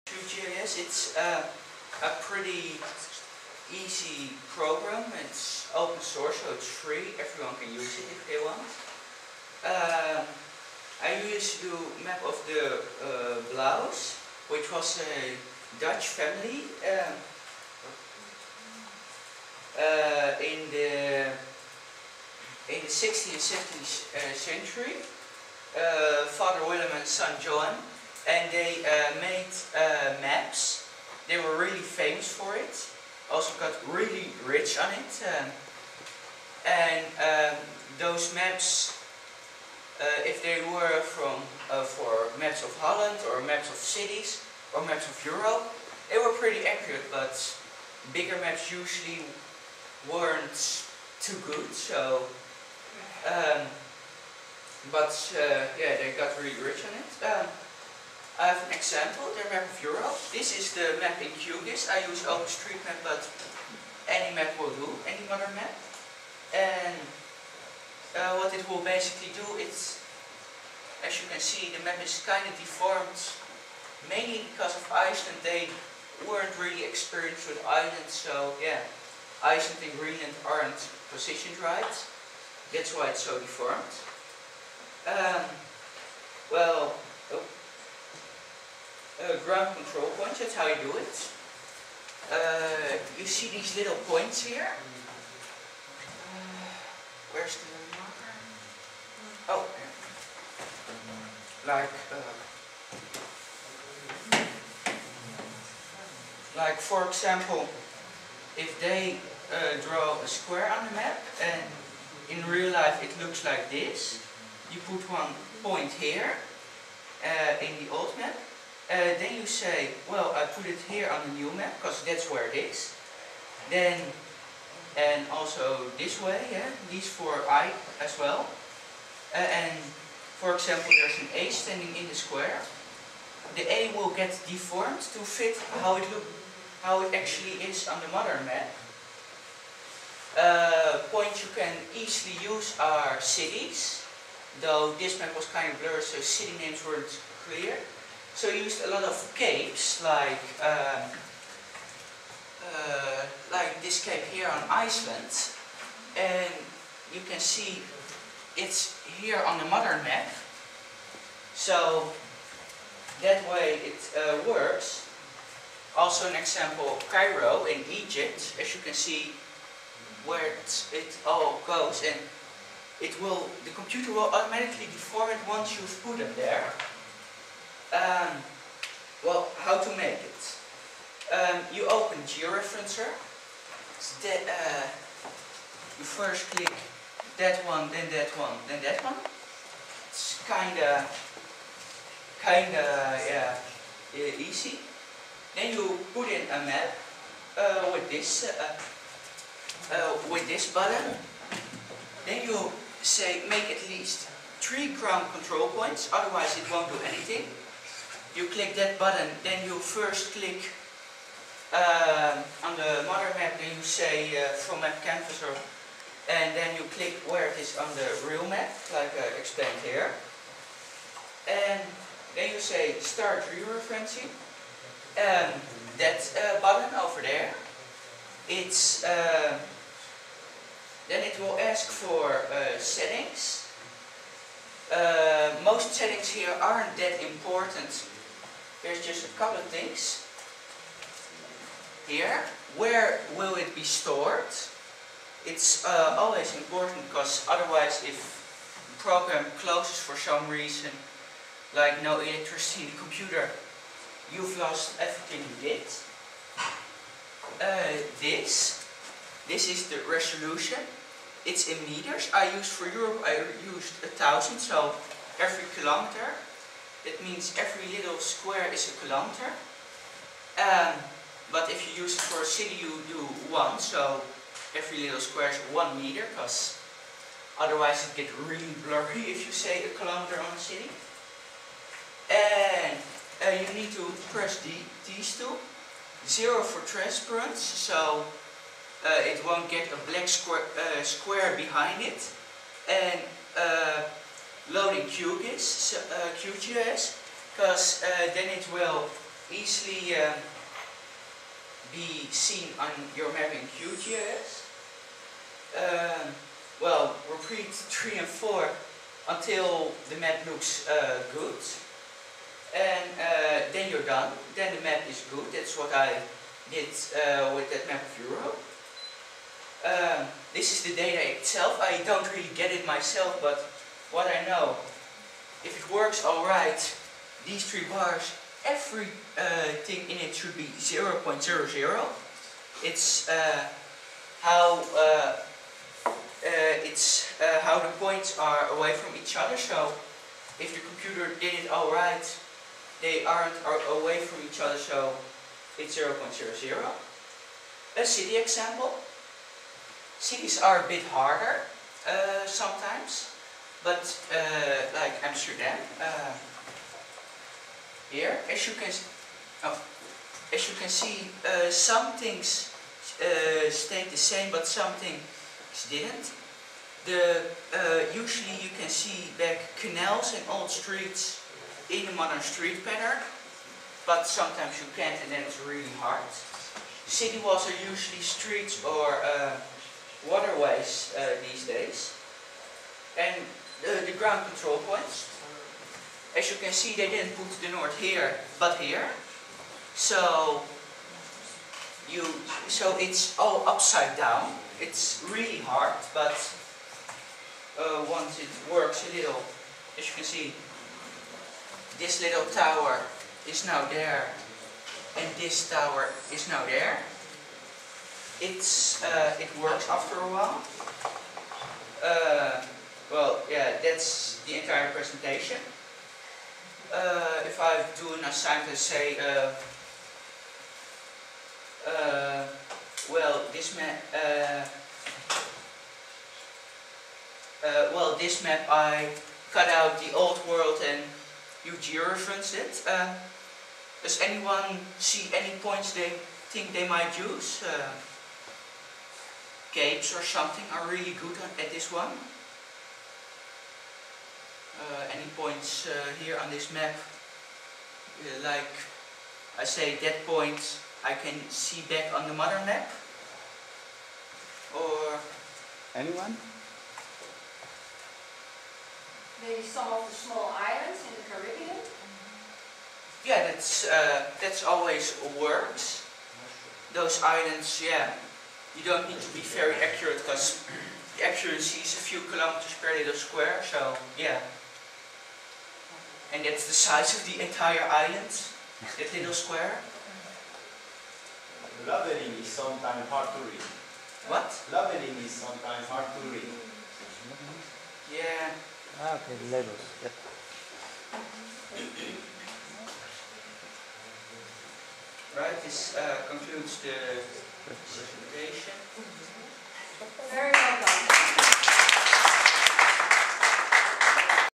Presentation about how to georeference a map of Blaeu with QGis
Presenation by a 15 year old about georeferencing maps of Blaeu with QGis.